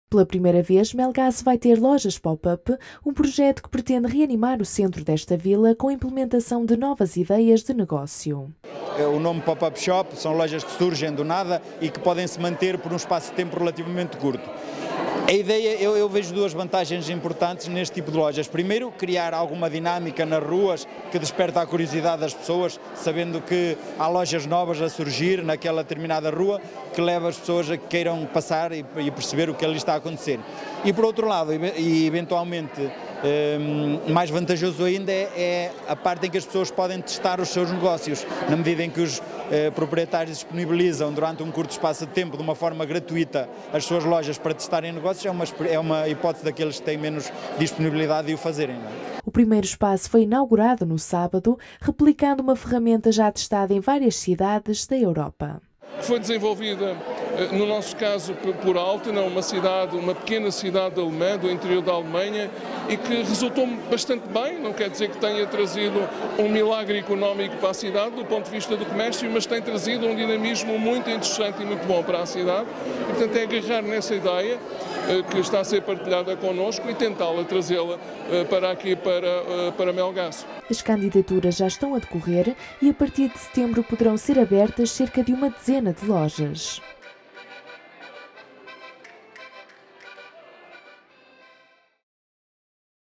Melgaço apoia criação de novos negócios com as primeiras lojas Pop Up | Altominho TV
As lojas Pop Up são um projeto já implementado em várias cidades da Europa e que pretende agora reanimar o centro da vila de Melgaço com novas ideias de negócio.